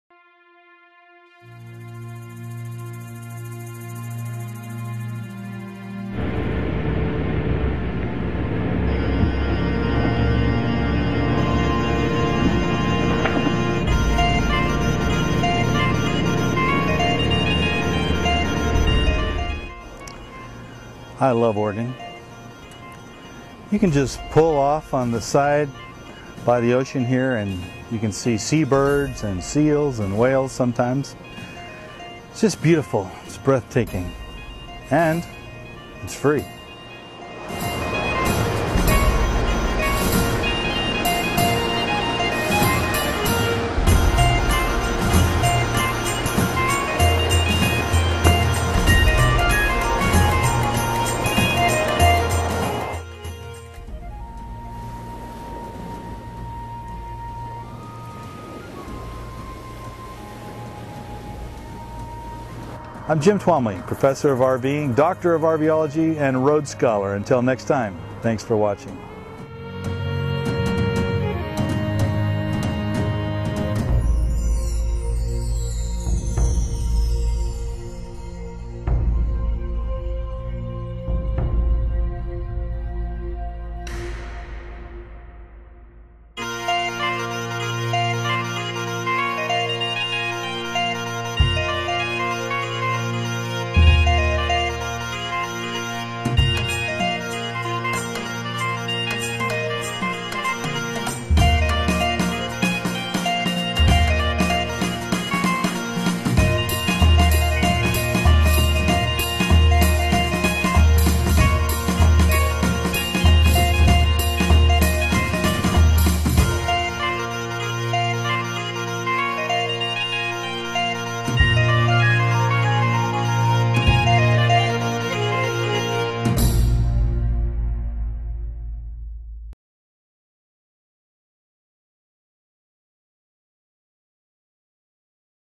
RV Table For Two WIth An Ocean View
Professor of RVing, Jim Twamley, takes you to the spectacular Oregon coast in his RV.